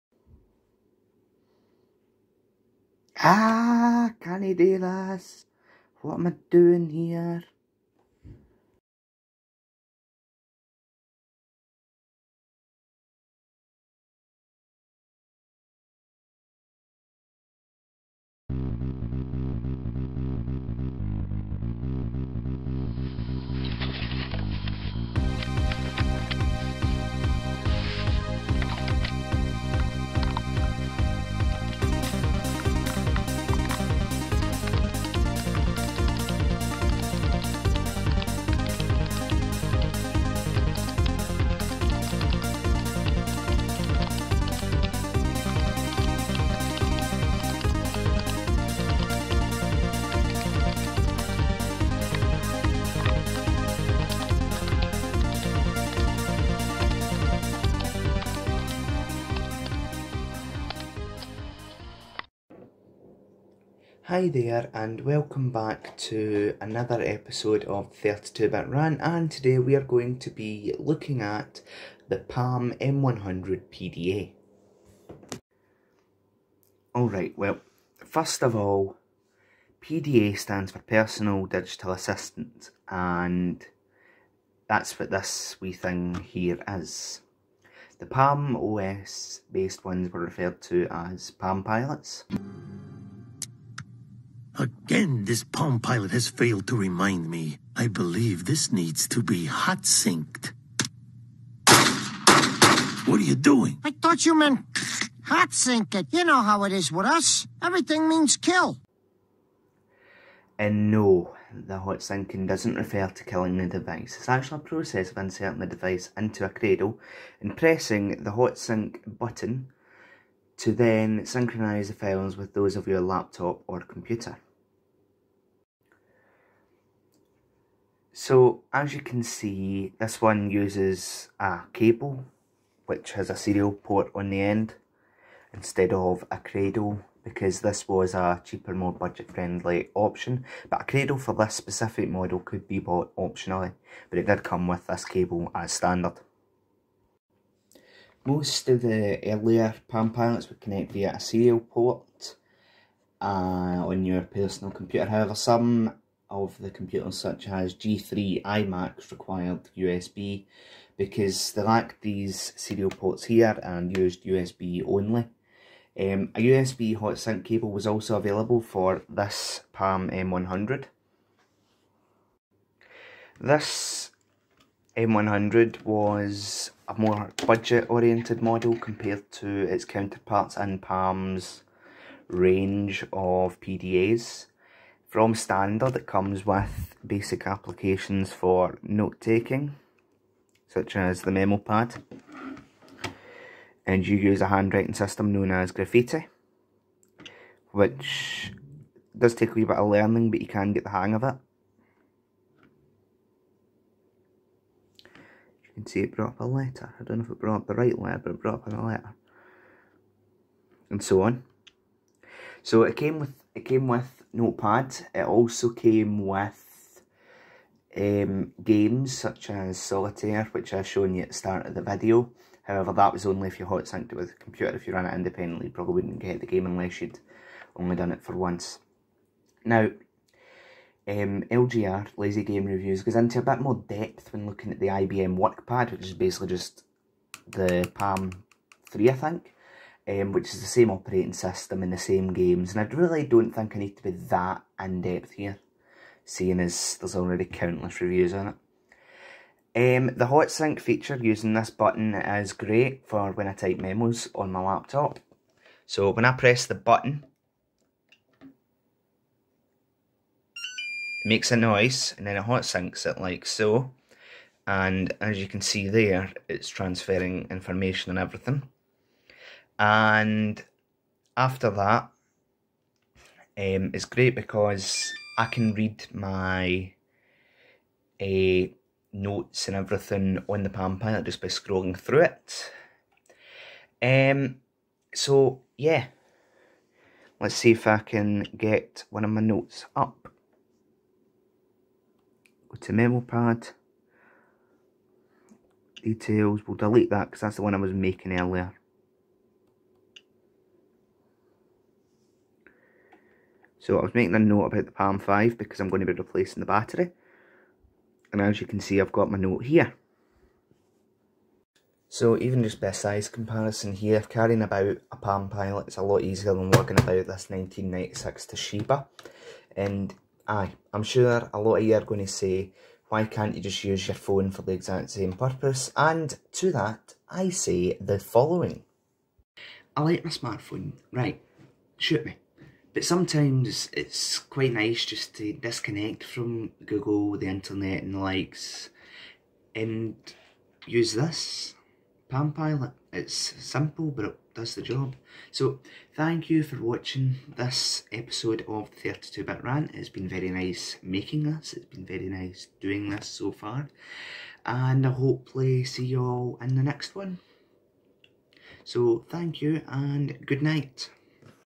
The Palm M100 PDA | Old Windows Malarkey
Welcome back to another video, today we looked at the Palm M100 PDA, also known as a Palm Pilot.

Yes, technically it's not OLD WINDOWS MALARKEY, but it is to an extent because I use my old Toshiba T2130CT running Windows 95 to hot-sync it.